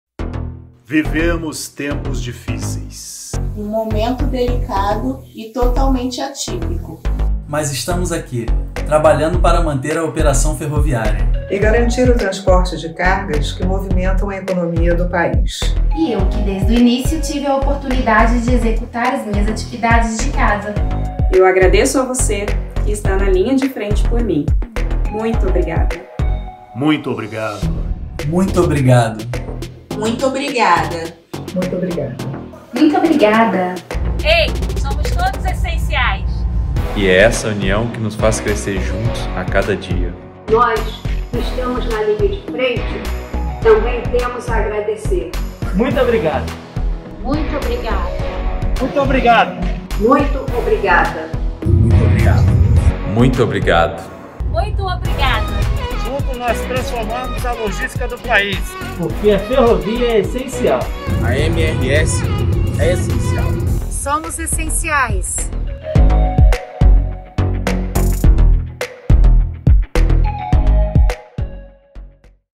Todos(as) somos essenciais!
O time da MRS é composto por ferroviárias e ferroviários que, por saberem que são essenciais para a logística do país, se unem por um bem maior.

Este vídeo é uma homenagem a todas e todos aqueles que, todos os dias, constroem o futuro da ferrovia no Brasil aqui na empresa.